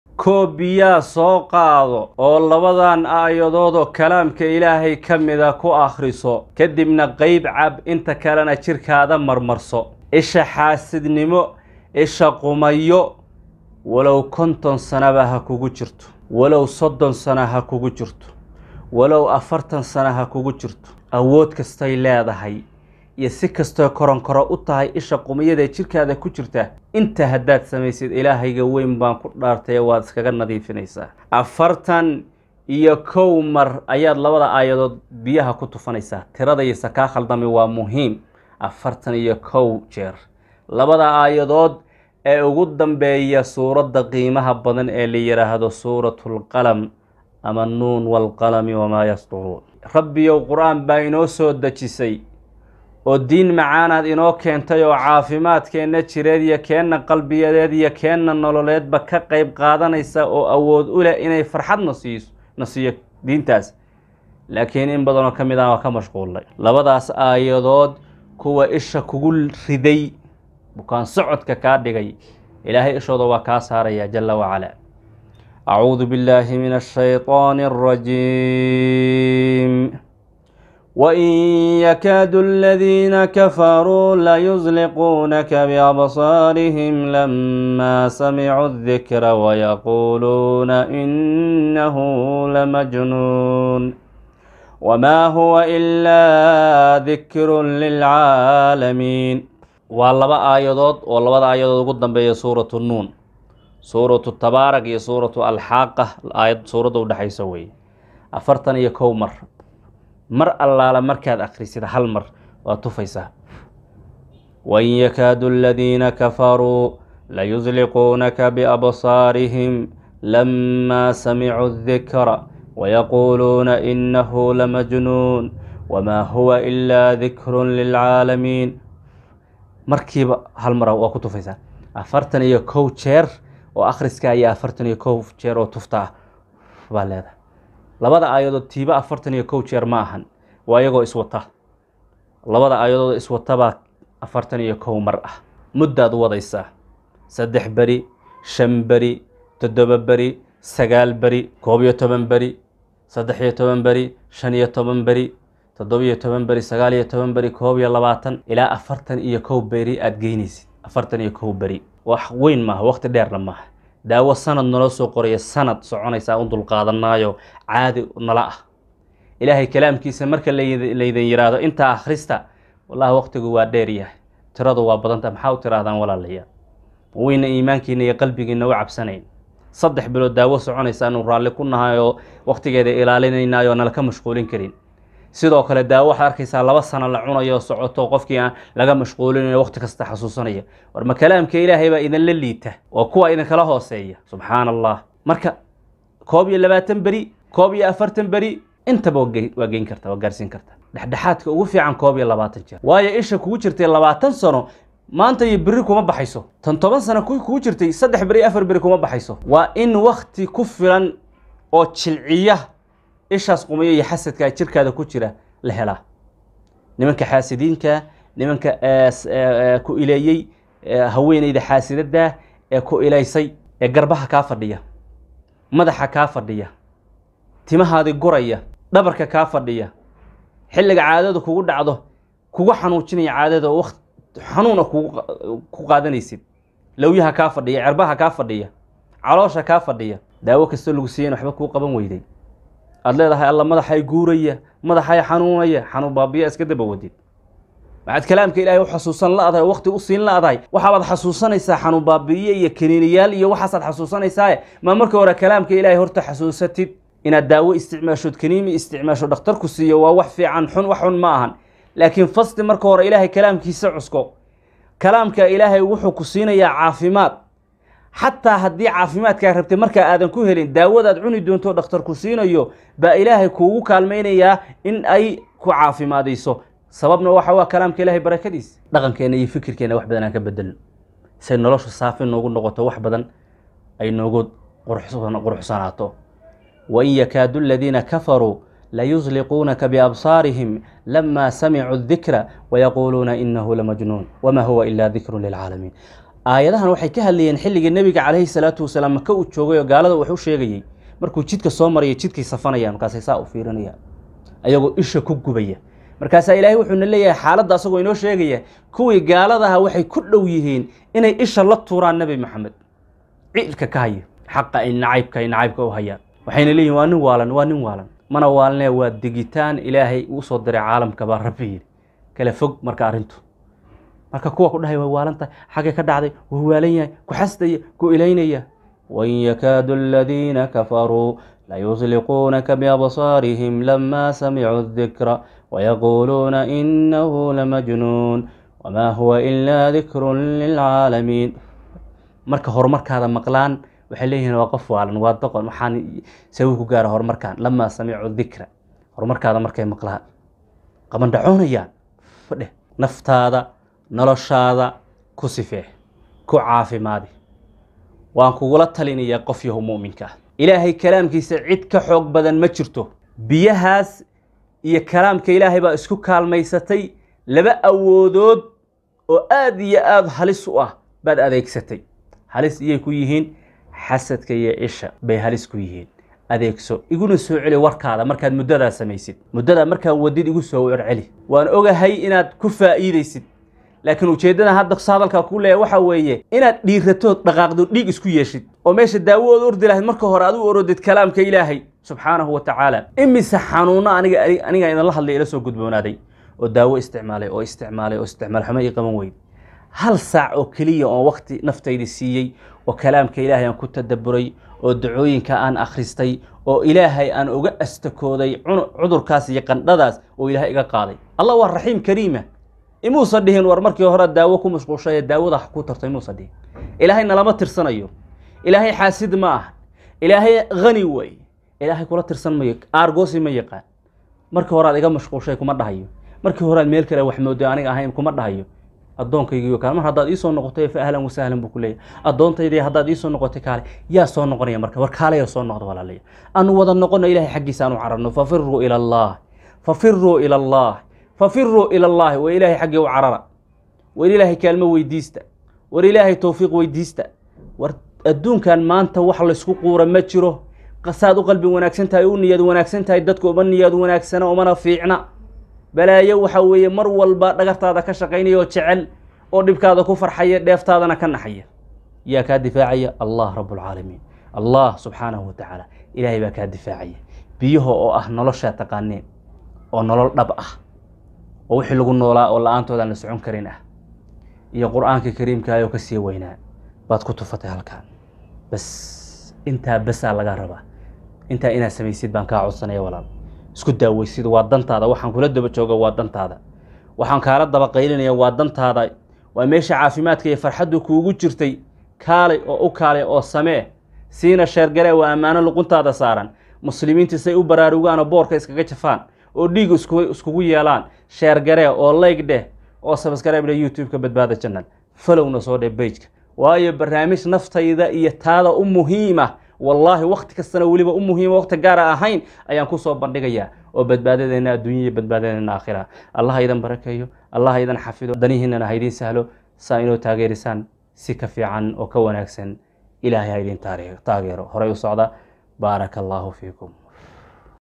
41 jeer 2 aayad ku Akhri Koob Biyaa isha Qumayo iyo Xaasidnimada Ka Saar jirkaada xitaa (100 sano)
41 jeer 2 aayad ku Akhri Koob Biyaa isha Qumayo iyo Xaasidnimada Ka Saar jirkaada xitaa (100 sano)